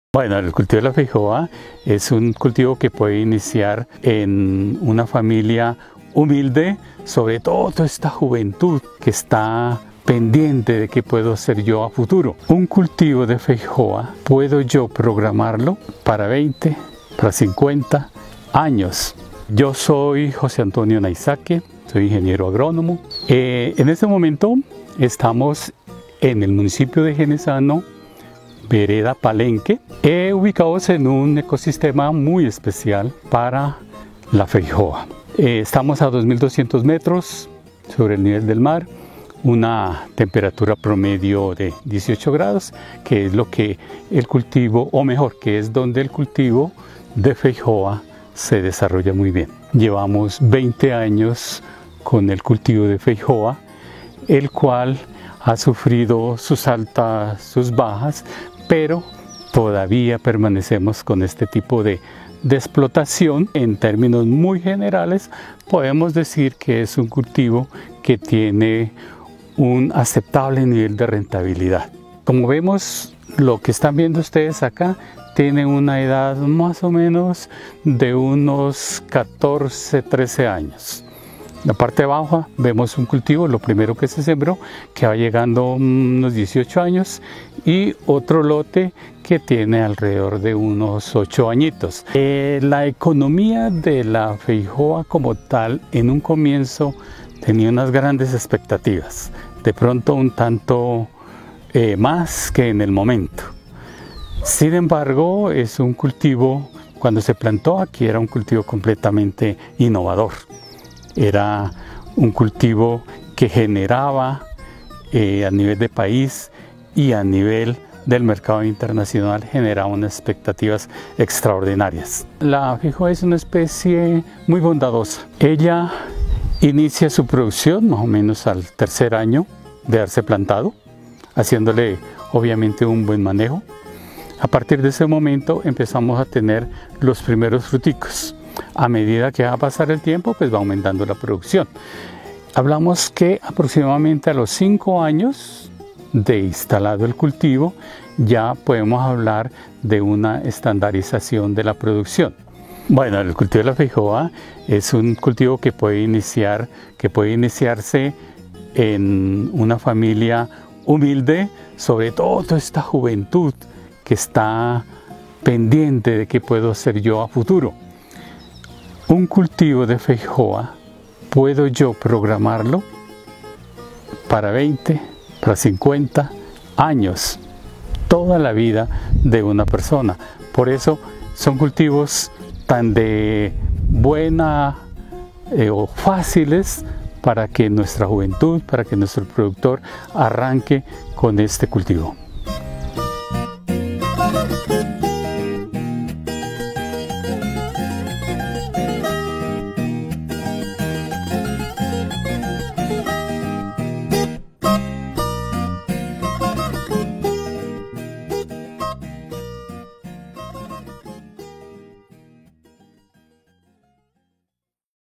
Producción y cultivo de de feijoa ❤️🚜 una alternativa que mueve la economía.
Producción de feijoa ❤️🚜 una alternativa que mueve la economia de Jenesano Boyaca siendo ina excelente alternativa.